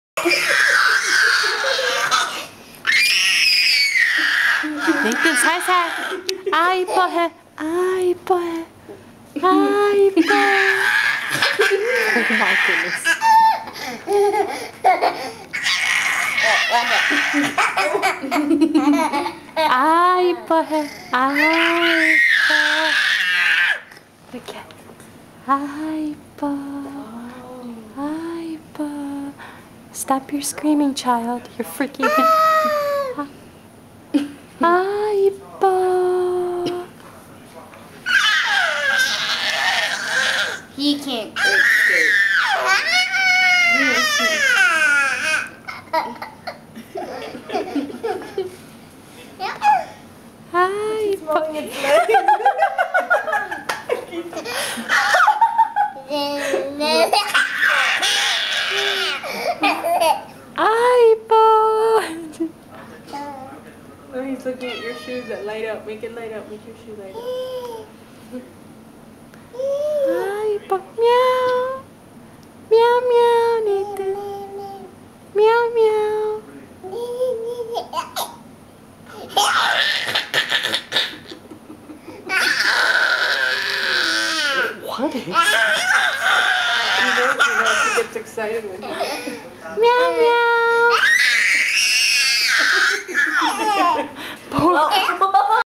Nathan Screaming/Laughing Freaking Out the Cat
Nathan loves animals and anything furry and/or soft. Watch him unable to contain himself!